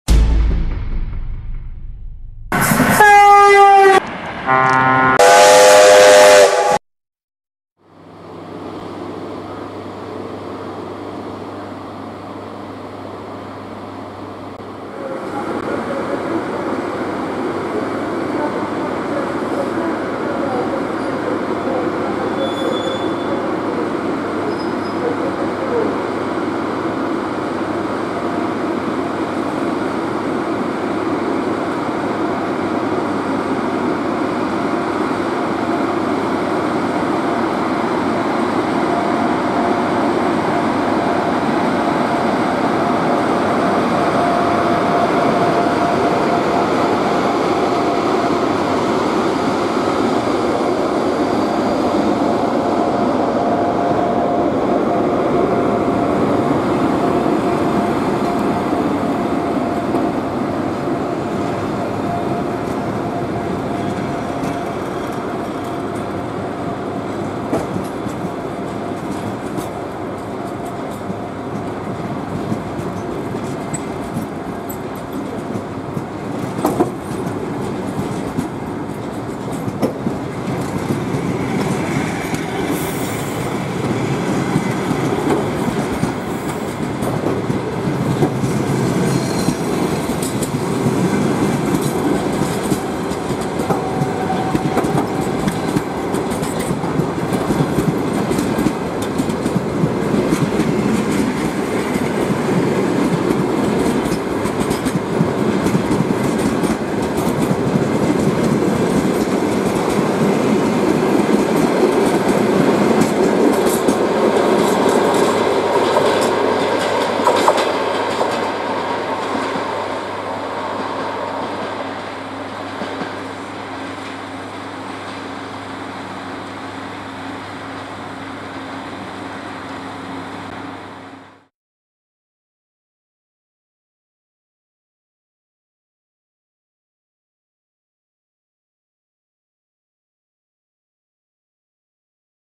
Отправление ЧС4-146 с поездом №141 Киев - Львов
Станция Святошино. Киев.